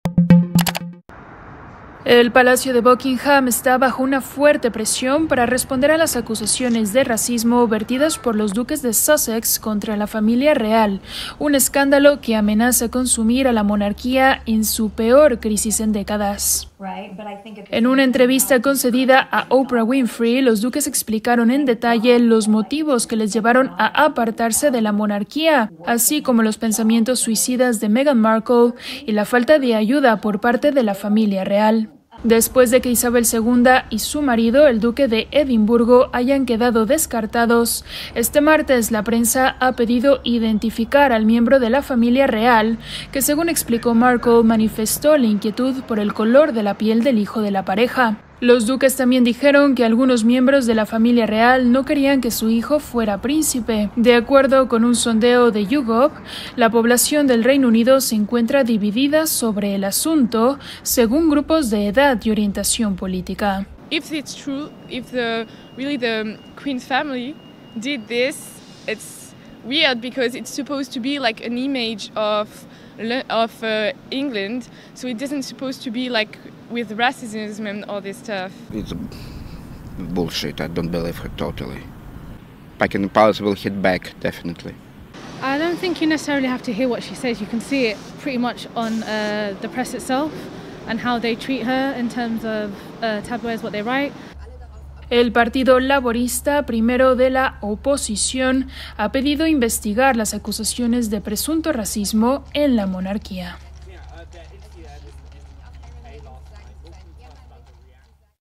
Buckingham, presionado ante las acusaciones de racismo
Londres (Reino Unido), 9 mar (EFE).- (Imagen: Clàudia Sacrest/Andy Rain) El palacio de Buckingham está bajo una fuerte presión para responder a las acusaciones de racismo vertidas por los duques de Sussex contra la familia real, un escándalo que amenaza con sumir a la monarquía en su peor crisis en décadas.